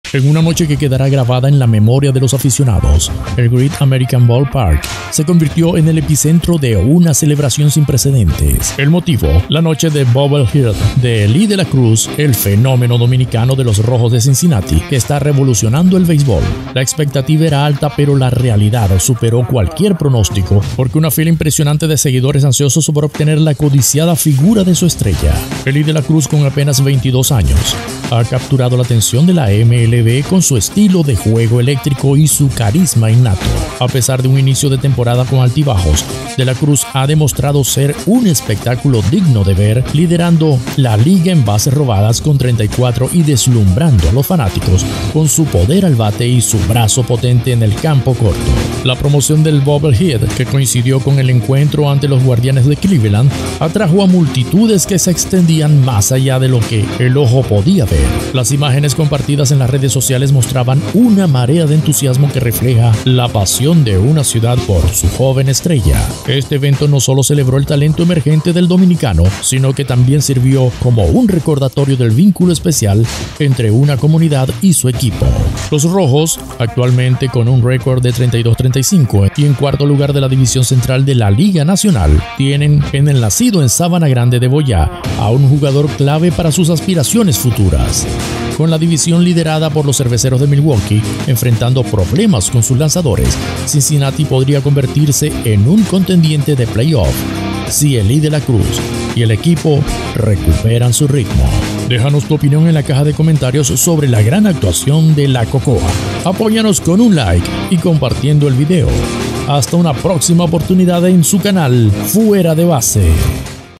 La LOCURA por Elly De La Cruz en Cincinnati
Elly De La Cruz, con apenas 22 años, ha capturado la atención de la MLB con su estilo de juego eléctrico y su carisma innato. A pesar de un inicio de temporada con altibajos, De La Cruz ha demostrado ser un espectáculo digno de ver, liderando la liga en bases robadas con 34 y deslumbrando a los fanáticos con su poder al bate y su brazo potente en el campo corto.